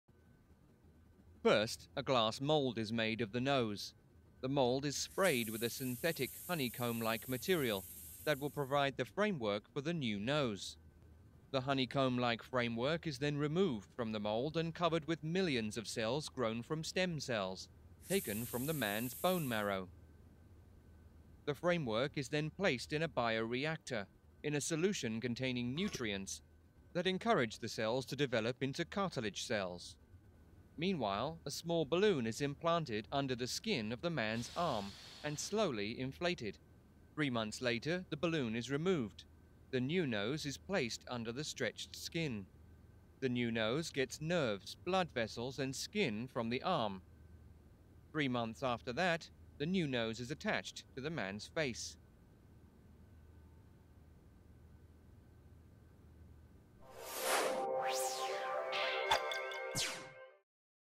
UK man has new nose growing on his arm
A British man who lost his nose to cancer is having a new nose made for him by scientists at University College London. The nose is currently being grown inside the man's arm, where it will acquire blood vessels, nerves and skin.
University College London professor Alex Seifalian told the Daily Mail that the new nose will look exactly the same as the old one. "His nose was a little bit bent to the left and we asked if he wanted it straight but he said no, he wanted it exactly the same," Seifalian told the Daily Mail.
This animation illustrates how the University College London scientists created the new nose from stem cells taken from the man's cartilage. SOURCE: Daily Mail